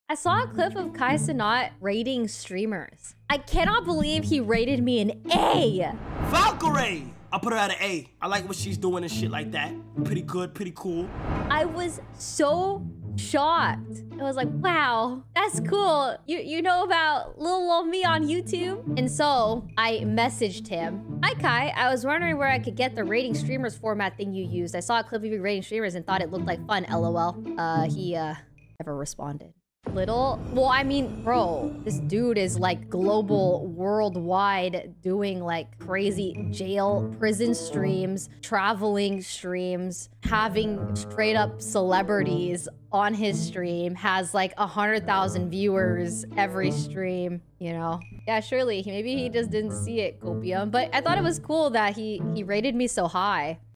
Valkyrae Reacts to Kai Cenat RATING HER
Use code 'Rae' to get your own MAVIX chair & $55.55 off